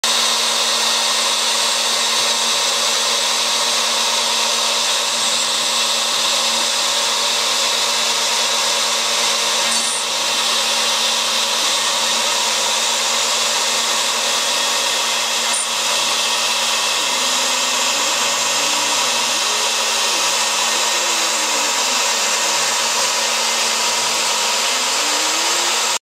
My Machine 2.AVI
Here is the sign a little farther along.. You can see that it takes a small bite each pass..  but the results are worth the time..
I start the machine and then check on it every 10 min or so.